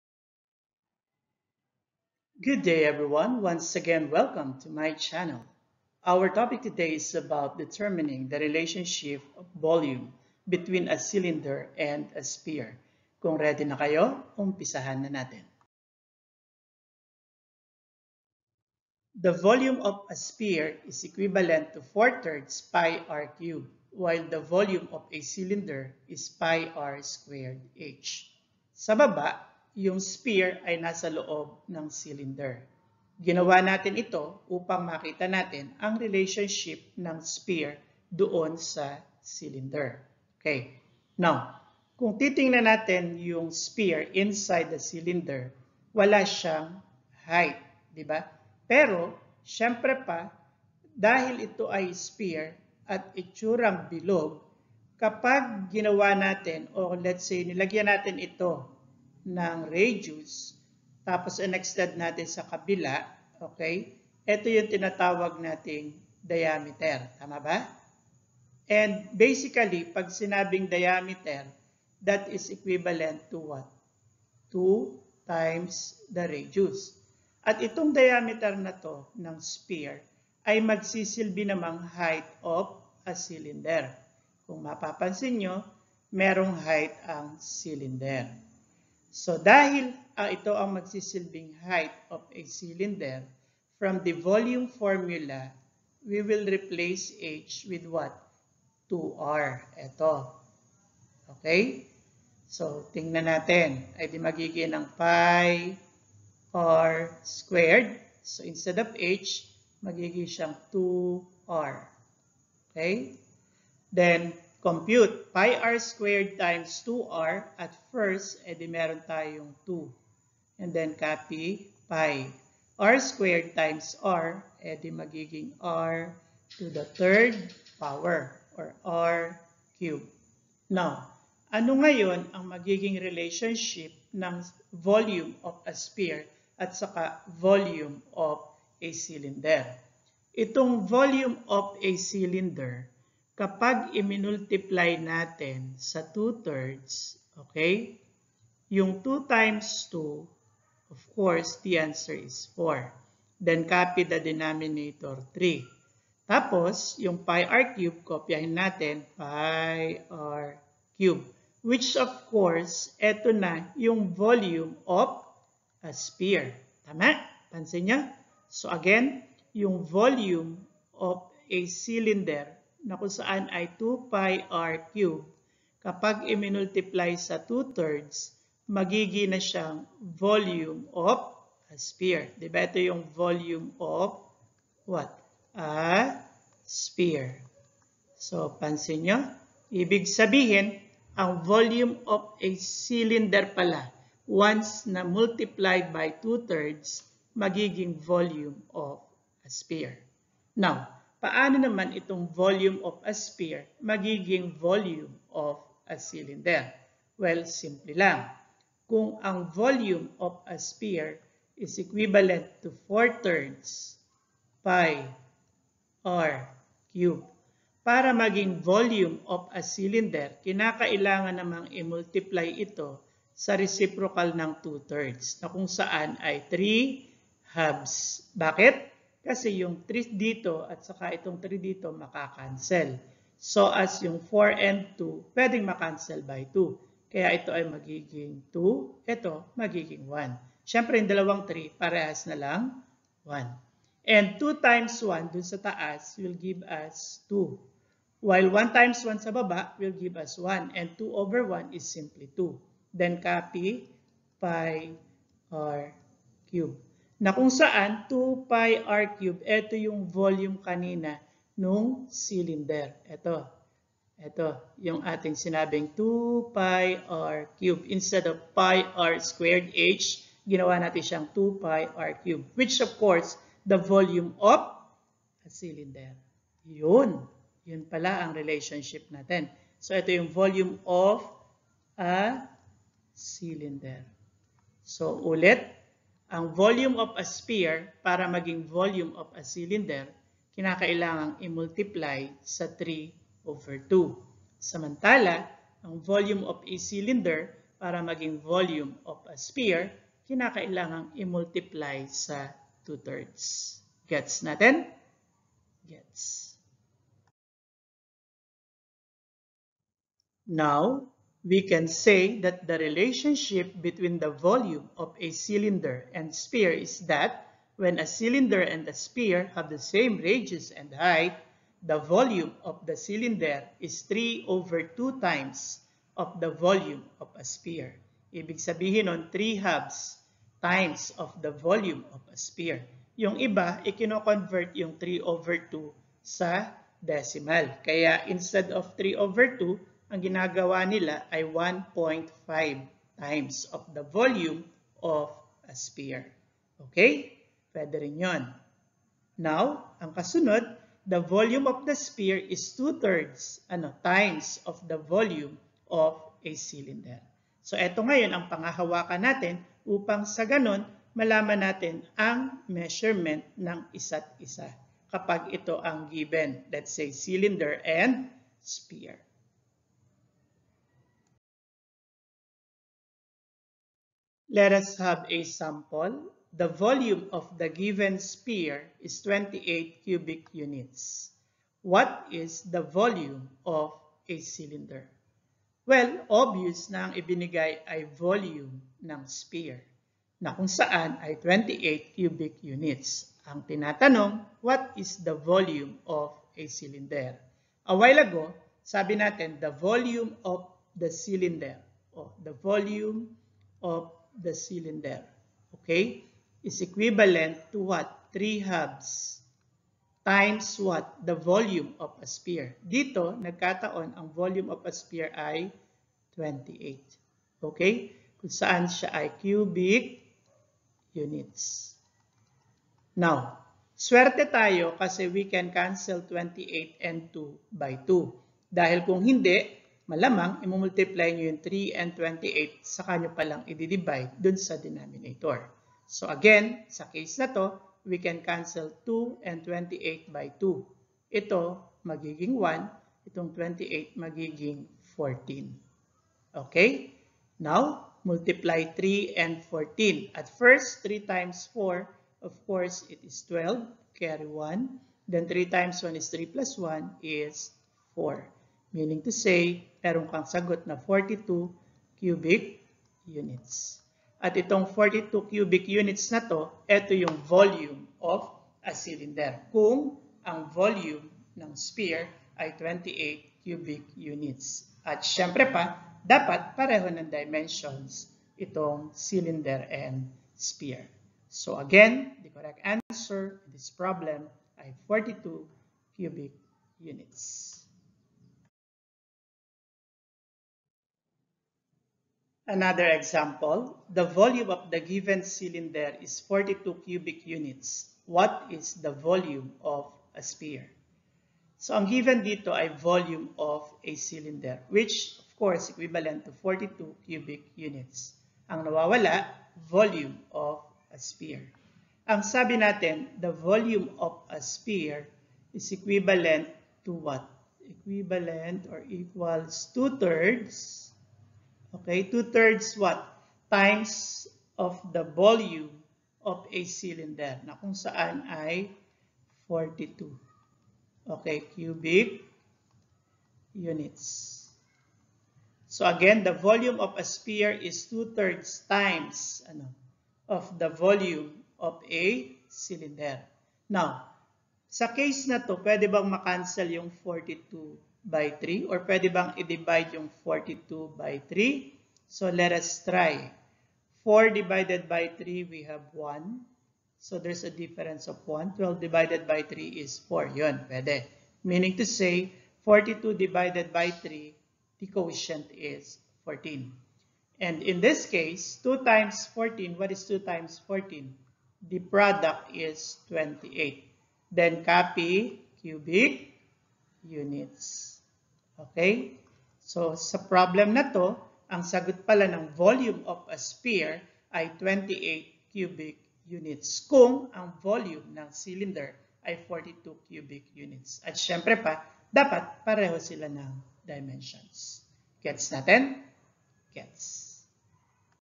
Determining the Relationship of Volume Between a Cylinder and a Sphere
Quarter 4 Mathematics 6
Week 1 LeaP Lesson

This video is about Determining the Relationship of Volume Between a Cylinder and a Sphere

The relationship between the volume of a cylinder and sphere is that when a cylinder and a sphere have the same radius and height…
  *  the volume of the cylinder is 3/2 times of the volume of a sphere
  *  the volume of the sphere is 2/3 times of the volume of a cylinder

Natalakay din dito ang mga sumusunod
  -  Volume Formula of a Sphere
  -  How the given height is converted to 2r